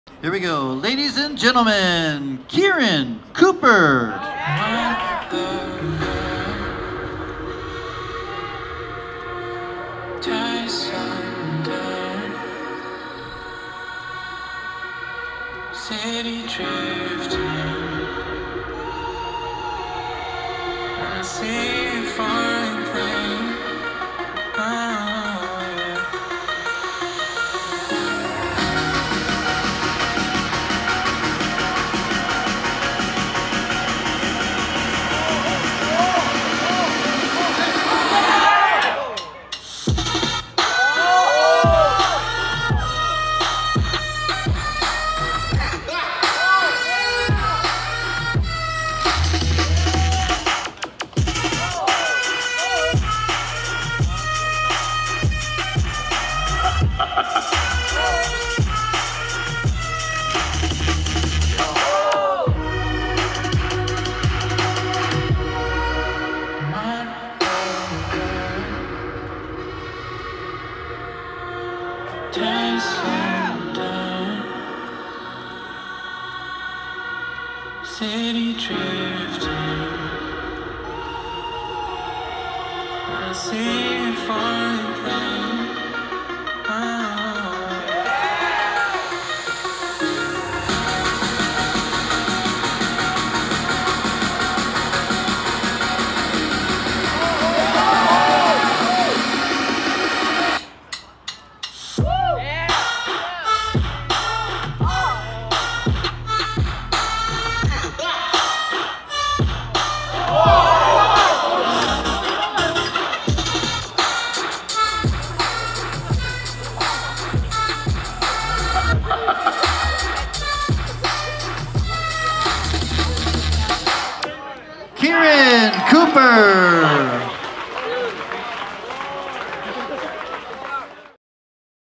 Keiran Cooper - 1A Final - 17th Place - BAC 2017 - Presented by Yoyo Contest Central
The 2017 Bay Area Classic Yoyo Contest was held on June 3rd at the Children's Discovery Museum in San Jose, CA. Presented by the National YoYo League, this event was made possible by the following sponsors:

JD Events